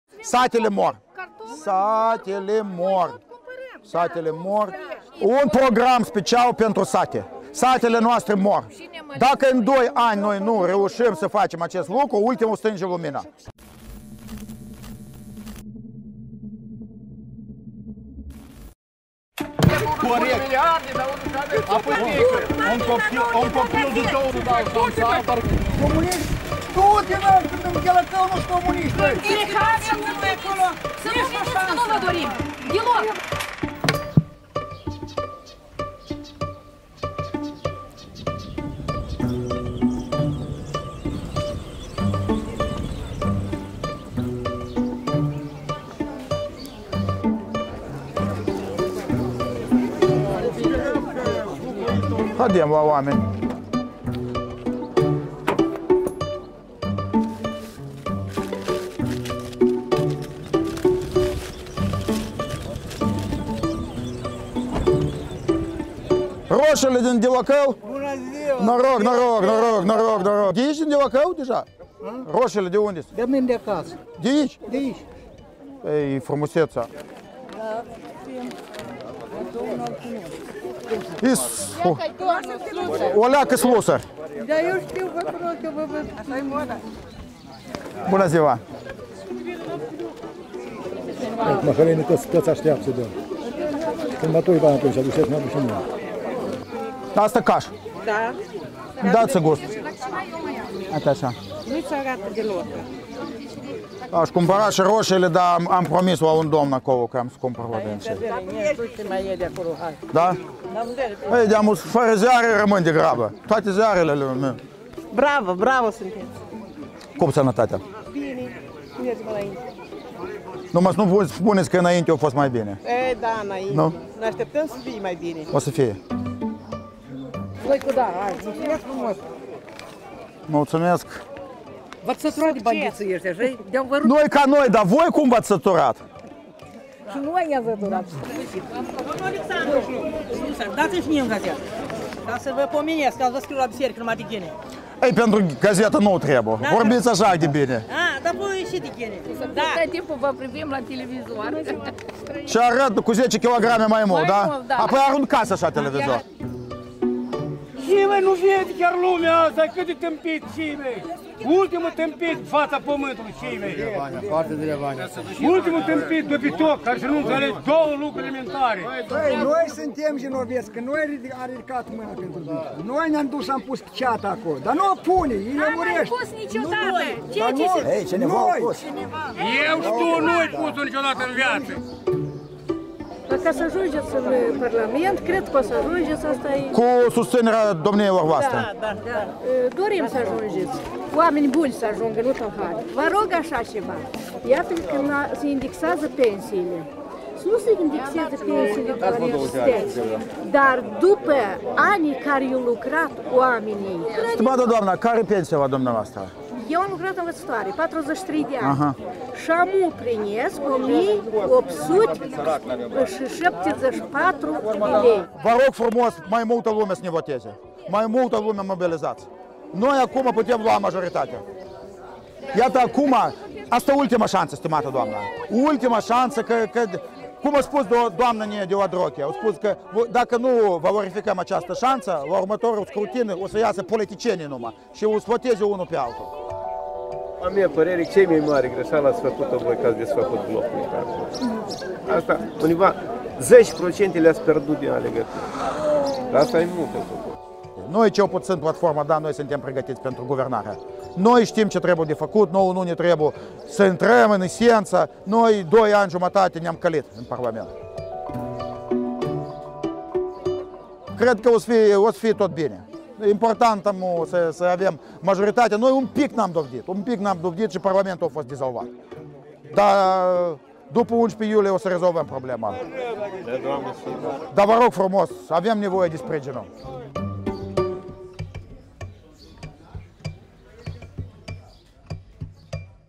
Platforma „DA-da-da, votați-ne!”
Cu câteva zile înainte de încheierea campaniei electorale, activiștii Partidului Platforma Demnitate și Adevăr, în frunte cu Alexandru Slusari, au ajuns în piața din satul Delacău, Anenii Noi.